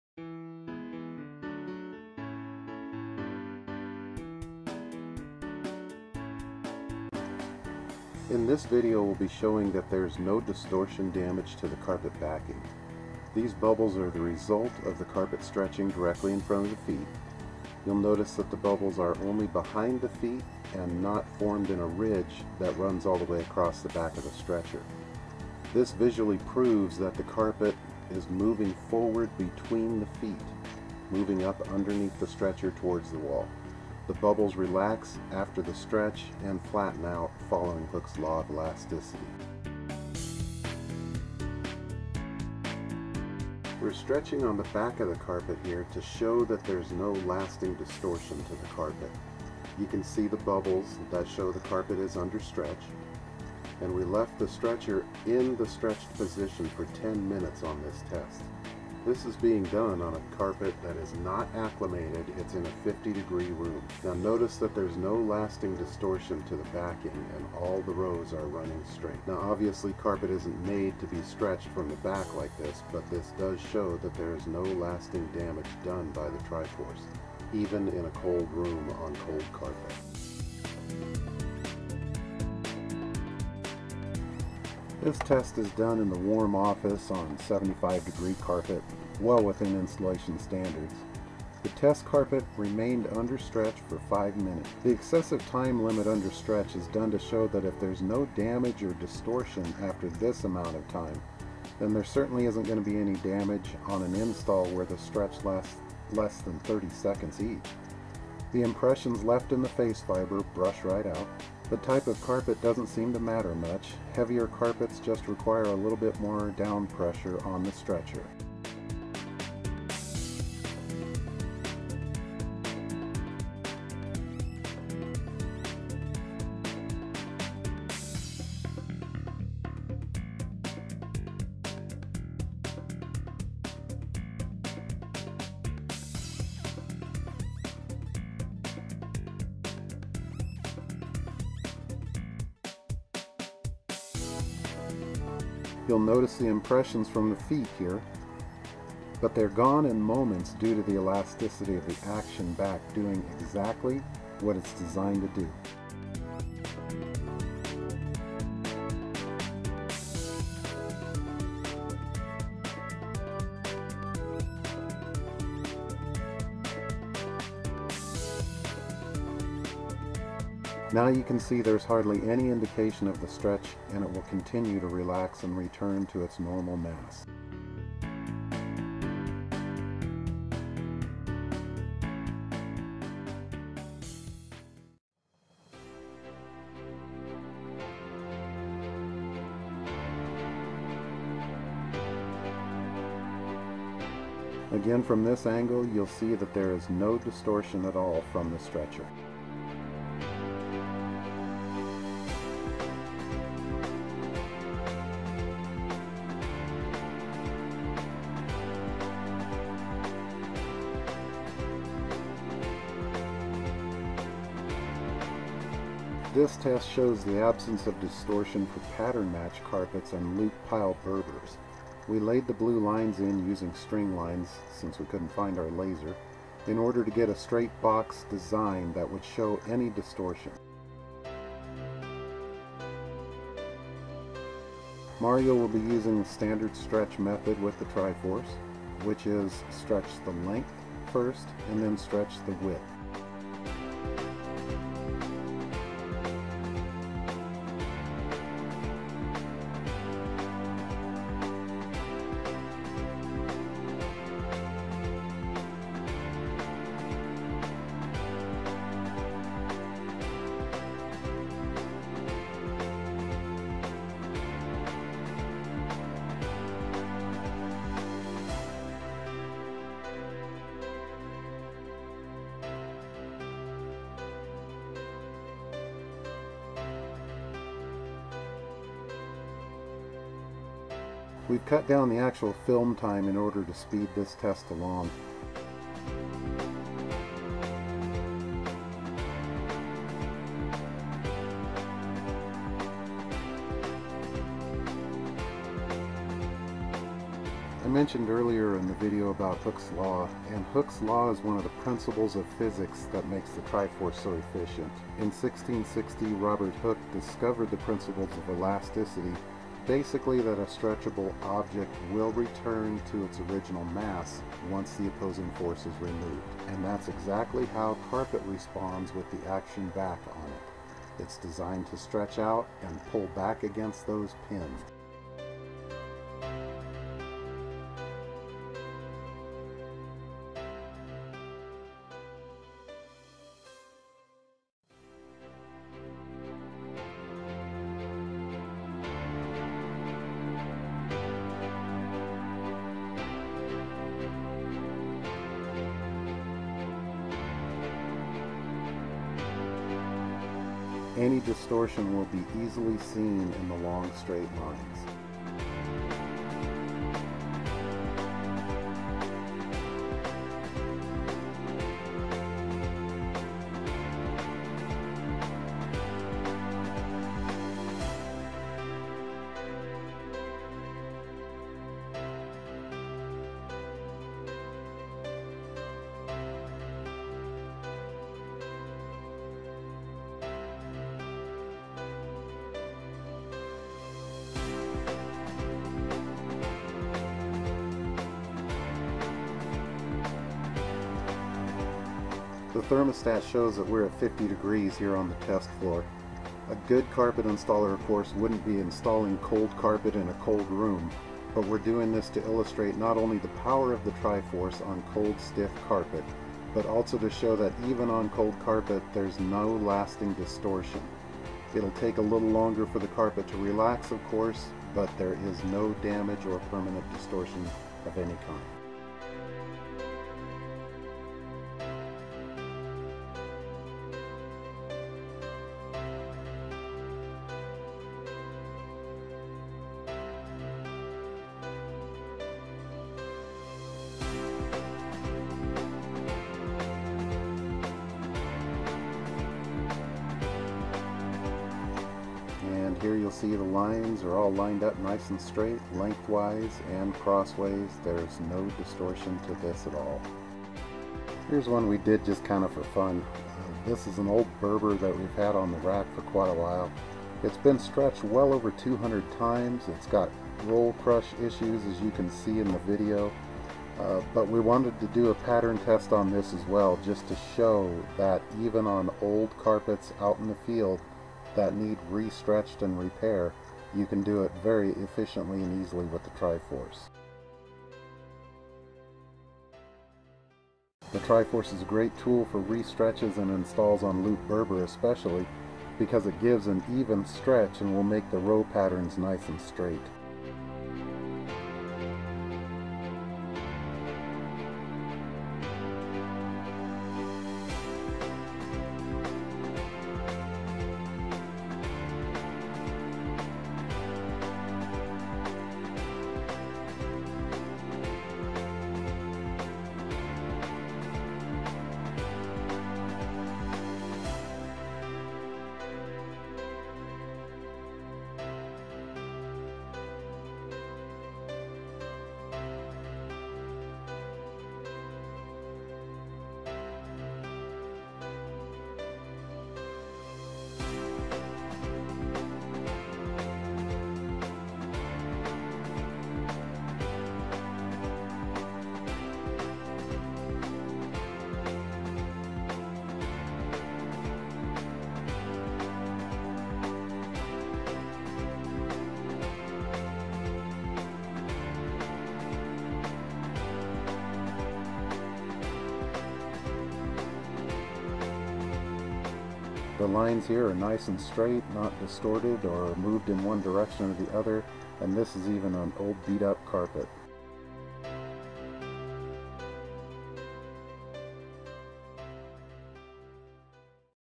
triforce testing
Third video in our series. The results of distortion testing are clearly seen in this videos and the results are spectacular.